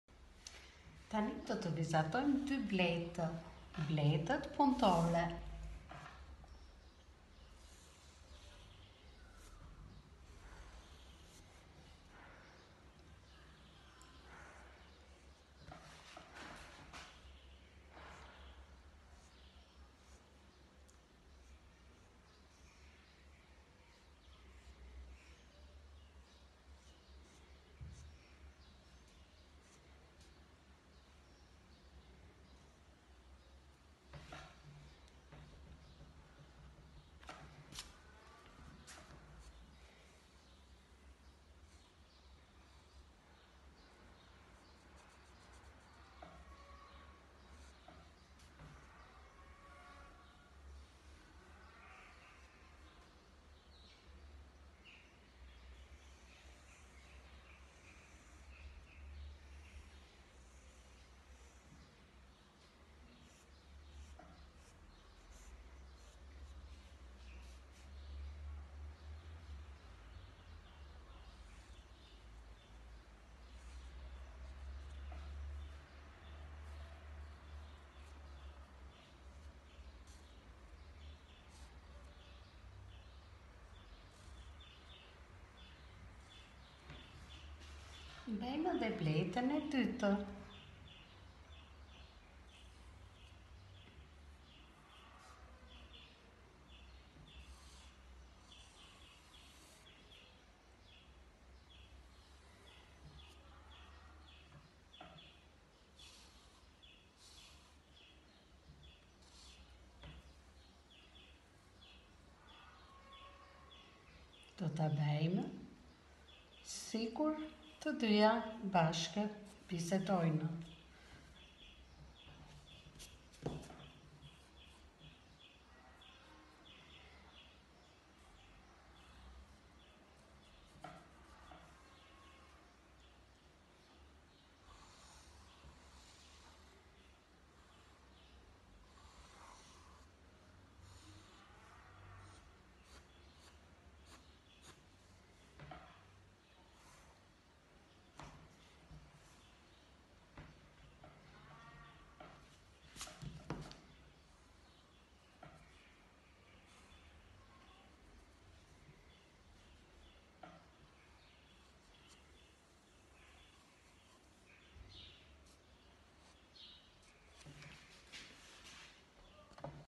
Si te vizatojme nje blete puntore | How to draw a bee 🐝
Sot do te vizatojme 2 bleta puntore te cilat po bisedojne me njera-tjetren.